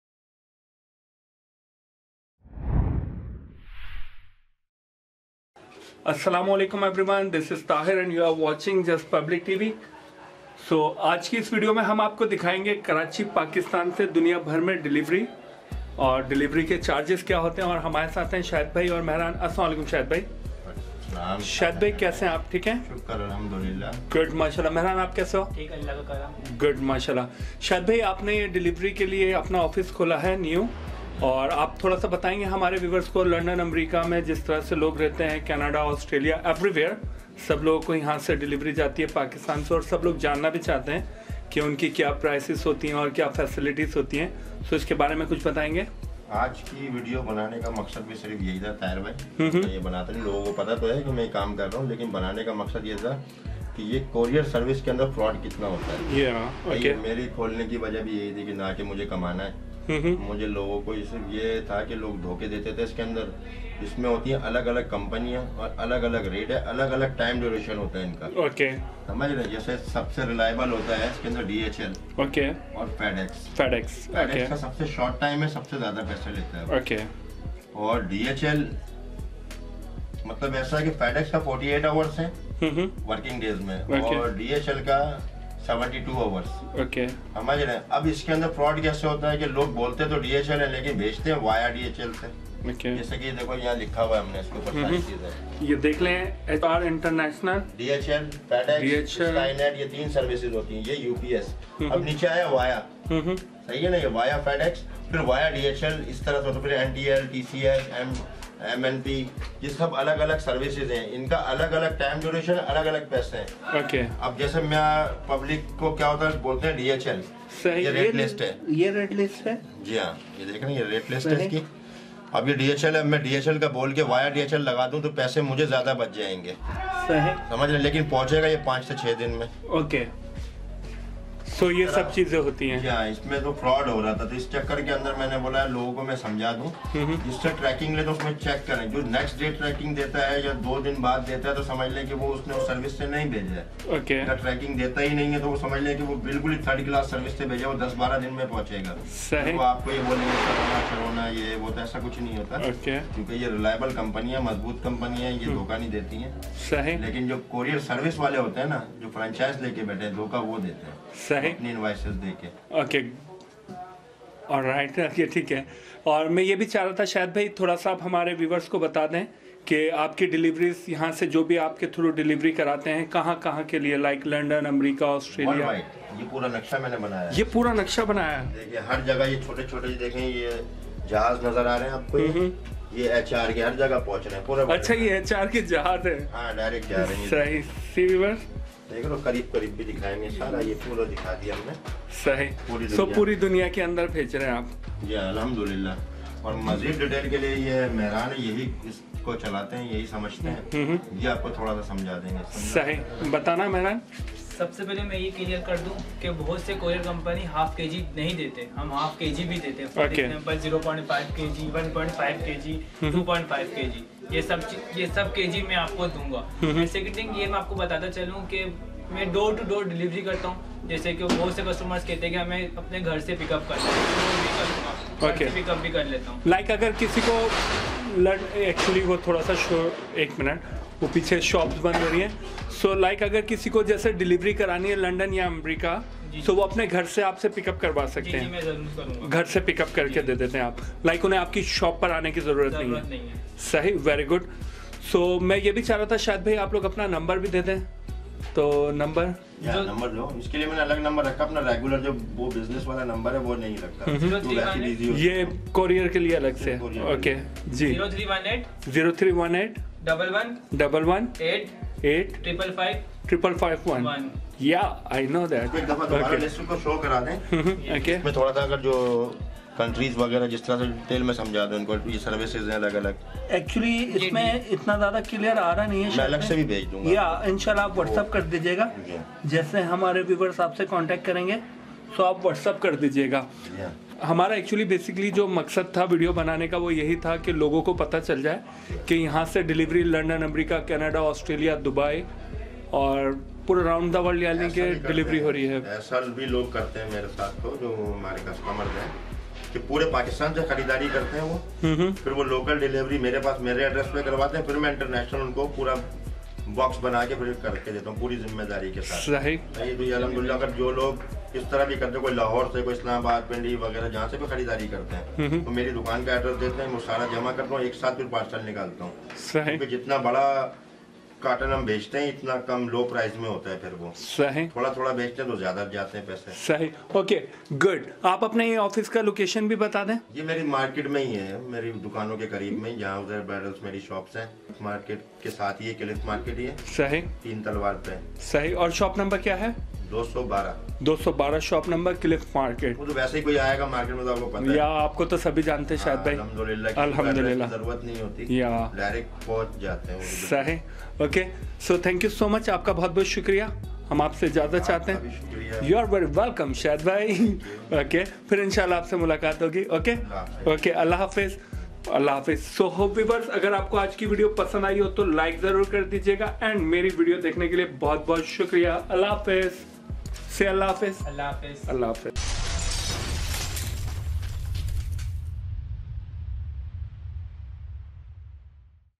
DHL FedEx Delivery Service From Pakistan To Worldwide - Get Online information
Hi everyone,
In this video I have explored DHL And FedEx Delivery Service From Pakistan To Worldwide.
Also you can get online information for your online booking parcel.

Call & WhatsApp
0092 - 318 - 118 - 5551